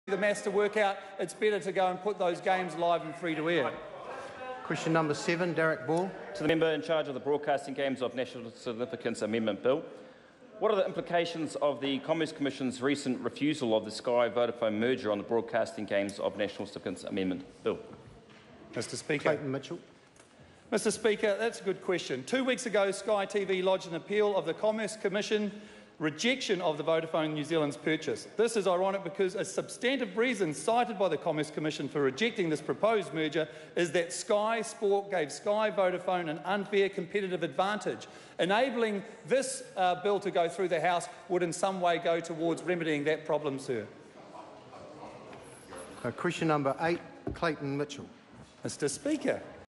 Question 7 - Darroch Ball to the Member in charge of the Broadcasting (Games of National Significa..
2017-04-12 - Question 7 - Darroch Ball to the Member in charge of the Broadcasting (Games of National Significance) Amendment Bill

Clayton Mitchell,Darroch Ball